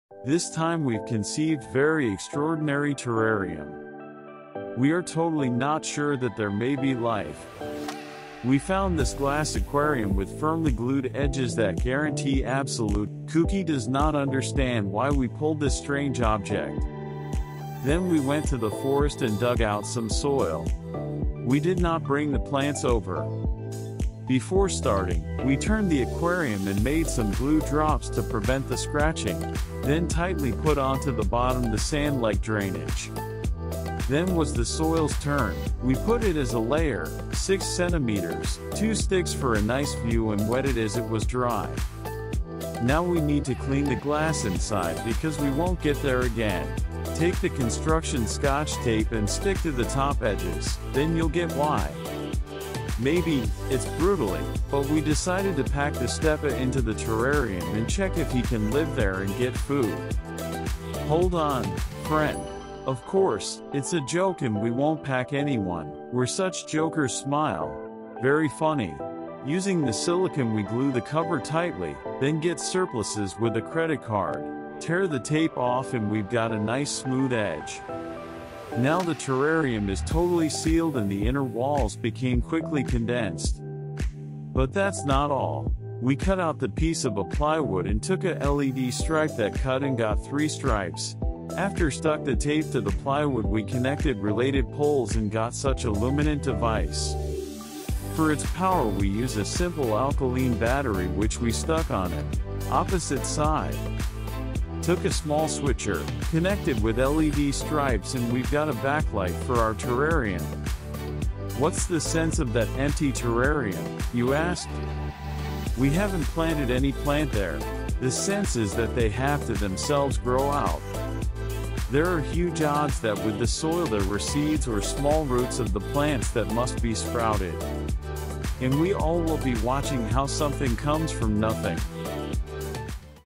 Creating a homemade terrarium with your own hands
In this video, we will begin a series of videos on creating a homemade terrarium with our own hands.
And we will also monitor how it develops and lives without human intervention

Happy viewing everyone! ;)